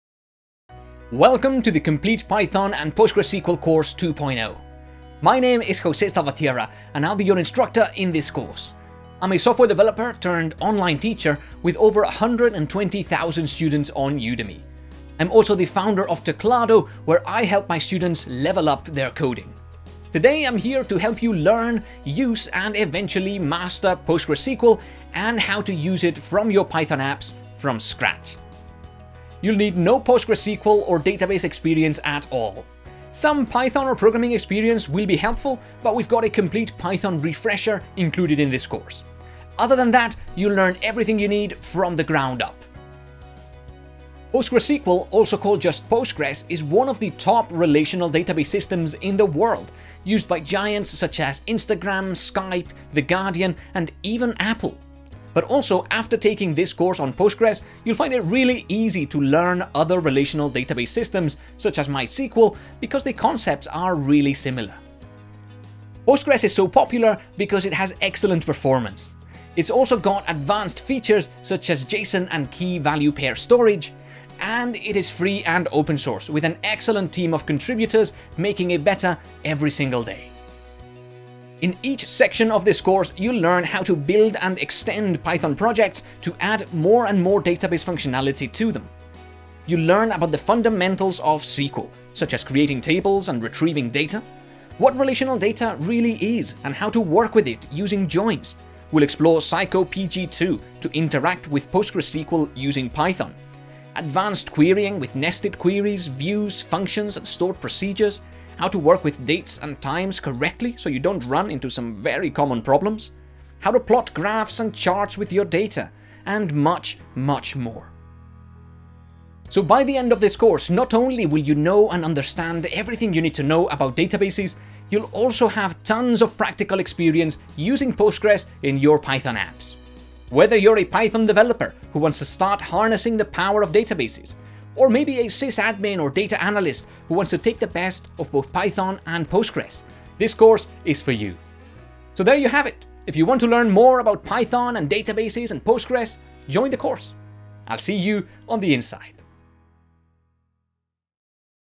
The Complete Python/PostgreSQL Course 2.0 by Jose Salvatierra
Learn to build your own apps in Python, and then improve them further by mastering PostgreSQL, in this brilliant course from renowned instructor Jose Salavatierra!

Across 15+ hours of content (and 9 practical projects!) you'll learn everything you need to master these two essential and modern technologies.

Jose Salvatierra is a software engineer, founder of software company Teclado, and online tech instructor who has taught over 130,000 students how to code in Python.